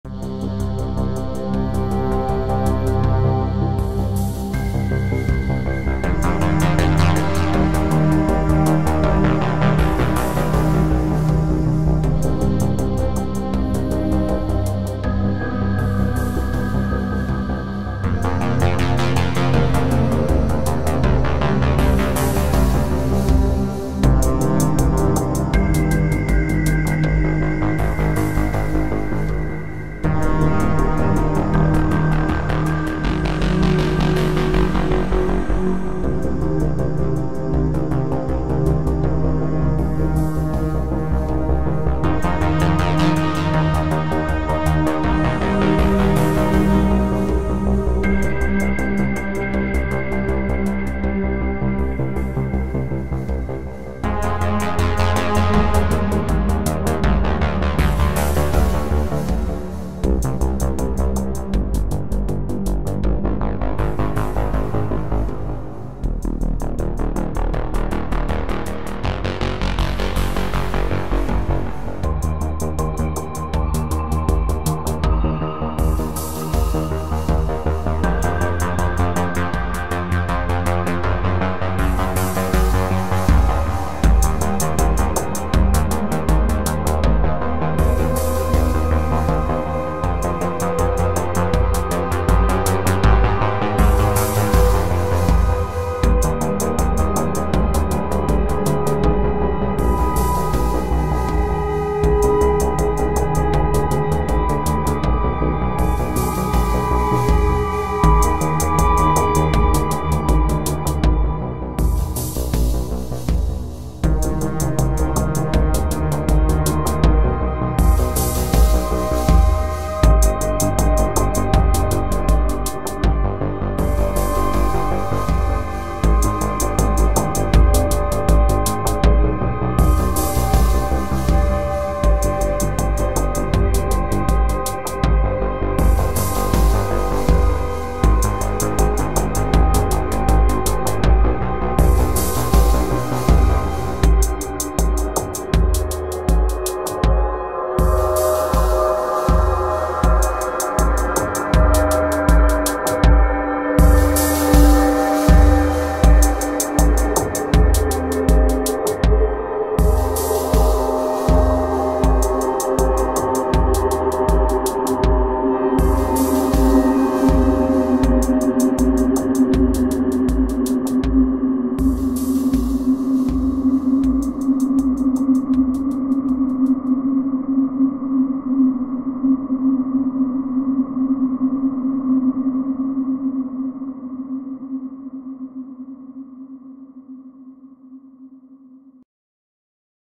Dark Ambient Bladerunner Music - dreamy synthwave cyberpunk
A slow burn ambient music video. My fascination with dangerous cunning femme fatales and sci fi music. Watch carefully and maybe you will experience a futuristic world of glamour and danger.